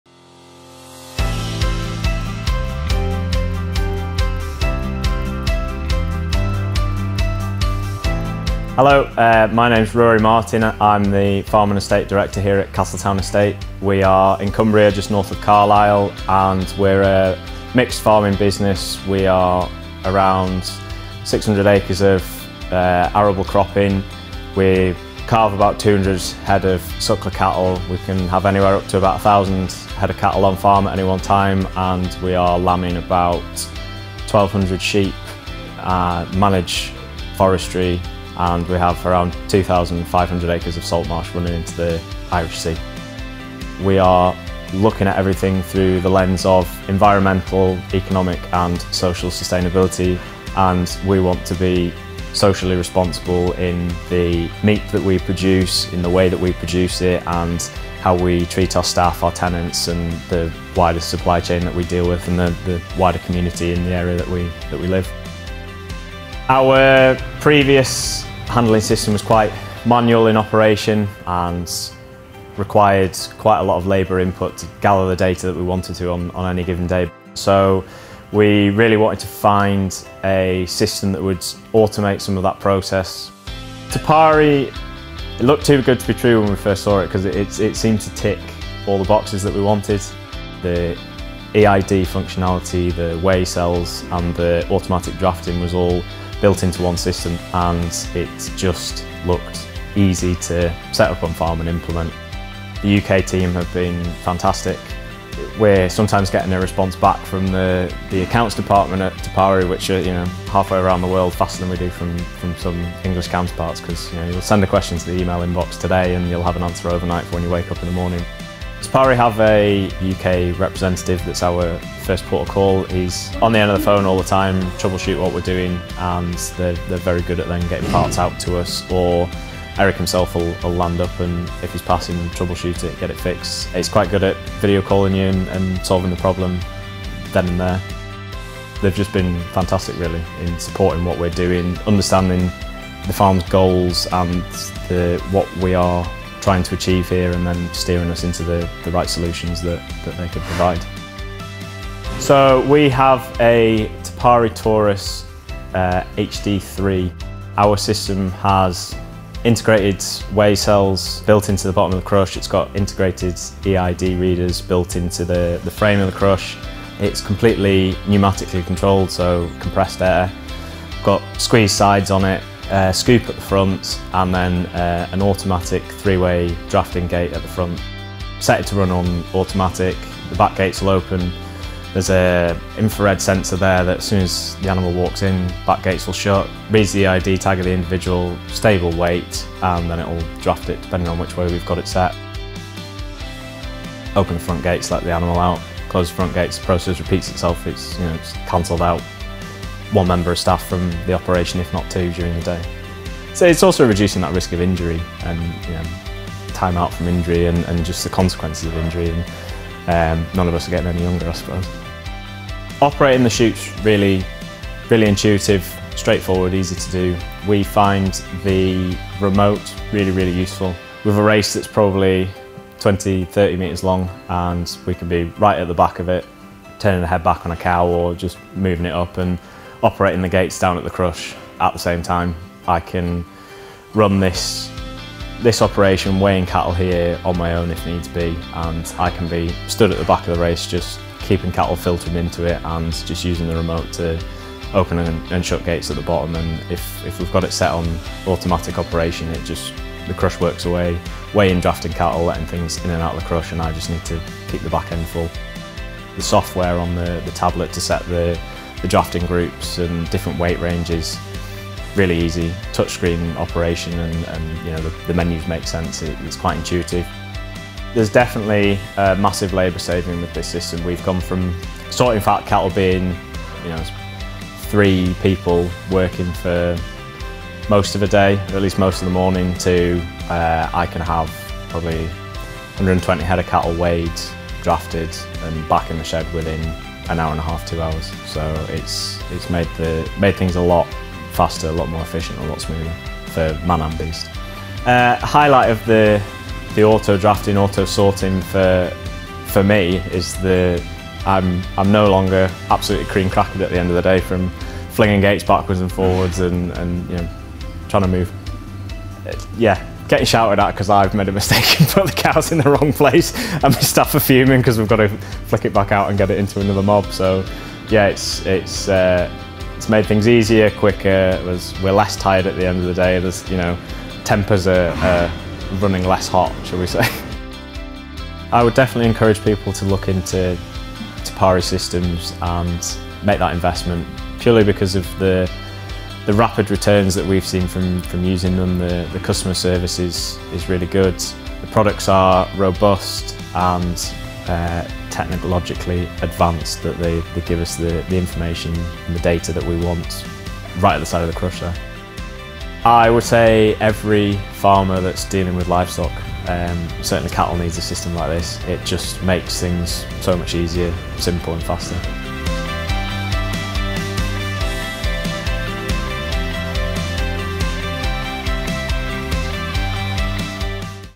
Easier Cattle Handling for Castletown Estate | Ruari Martin
Join Rauri Martin, Farm and Estate Director at Castletown Estate, as he talks to us about his experiences with the Taurus Auto Drafting Cattle Crush. With over 600 acres of arable cropping, 200 head of suckler cattle, 1,200 sheep, and 2,500 acres of salt marsh, Castletown Estate prioritize environmental, economic, and social sustainability. Seeking to automate their manual handling system, they turned to Te Pari and found the perfect solution with the Taurus HD3 Auto Drafting system. With weigh cells and Integrated EID readers, pneumatic control, and automatic three-way drafting gates, the Taurus streamlines operations while reducing labour and the risk of injury. Ruari says the support from Te Pari's UK team has been exceptional, ensuring quick responses and on-site assistance.
Experience the efficiency and innovation of Castletown Estate's farm operations powered by Te Pari's Taurus auto drafting system.

If you'd like to learn more about the Taurus Cattle Auto Drafter, please get in touch with our team.